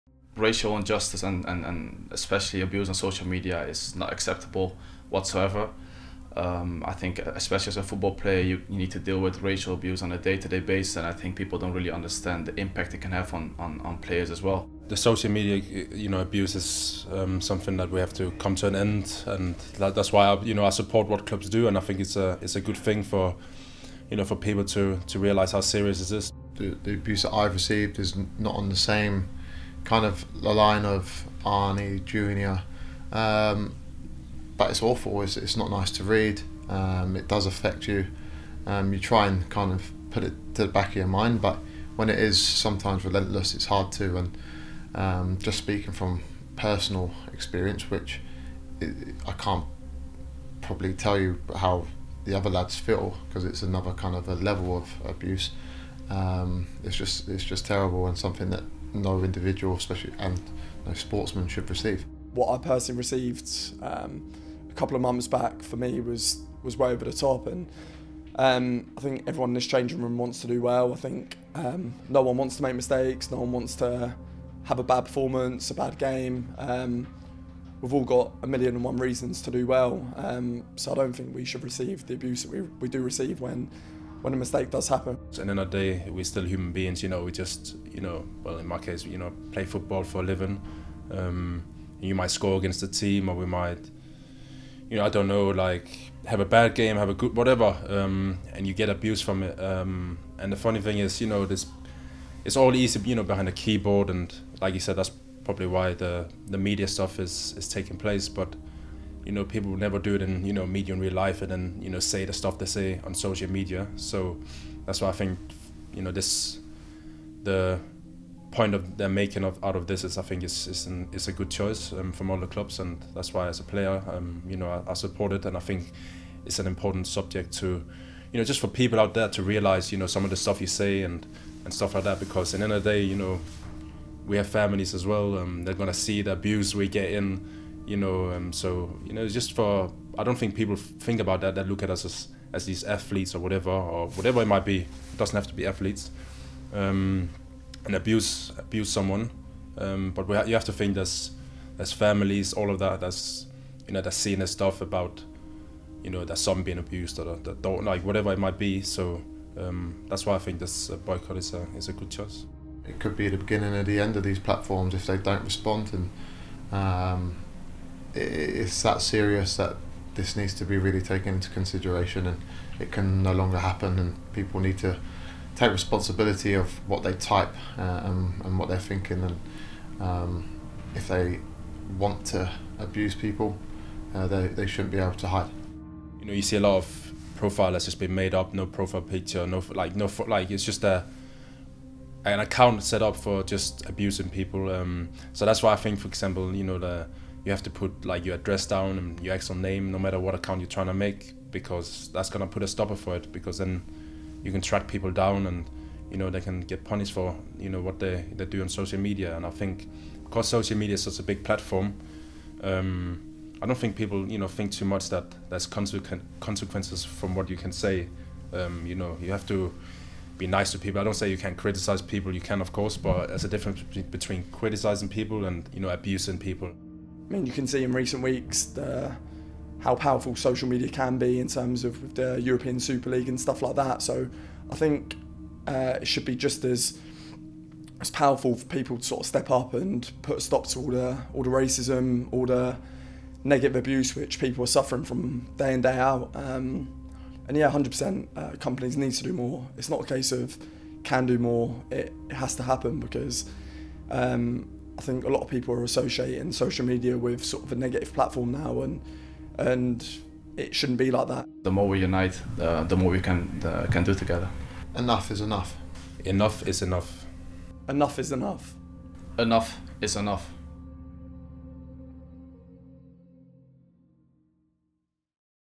We want change. Our players speak about online abuse 😠
This weekend AFC Bournemouth join a whole host of organisations, teams, athletes and teams in boycotting all social media platforms to make them put stronger preventative and takedown measures in place to stop discriminatory abuse being sent or seen.

Our players tell us about their experiences straight from the heart.